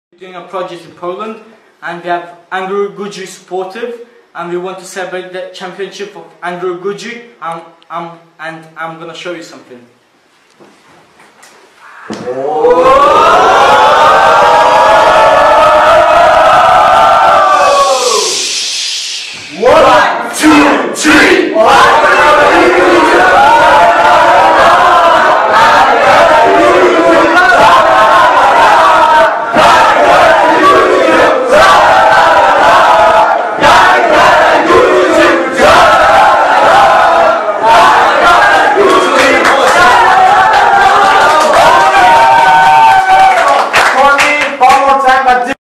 POLONYA ERASMUS PARTİSİNDEN ANKARAGÜCÜNE SEVGİLERLE!
Üniversite'nin gençlik değişim programı (erasmus) ile Polonyaya eğitim görmeye giden bir Ankaragücü taraftarı arkadaşları ile birlikte destek mesajı çekiyor.